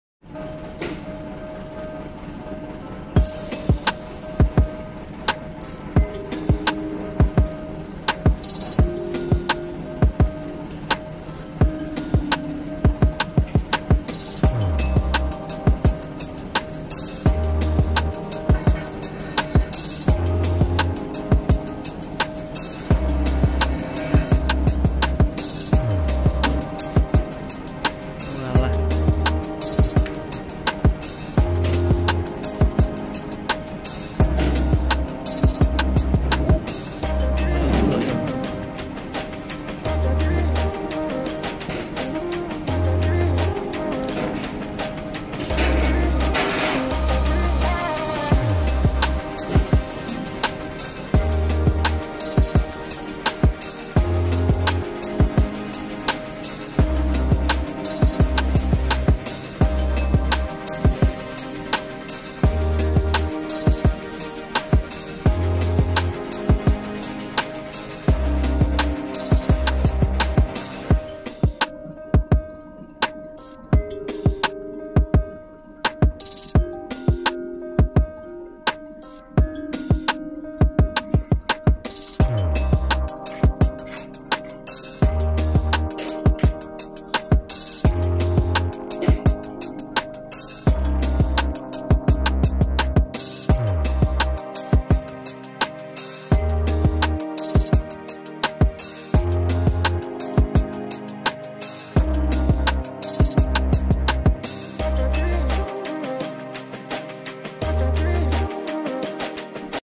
Cara menurunkan sepeda dari truk
Adalah video tanpa narasi untuk hiburan semata.
Truk yang digunakan sudah ada liftnya untuk menurunkan unit sepeda motor.jadi cukup mudah dalam proses penurunan sepeda motornya.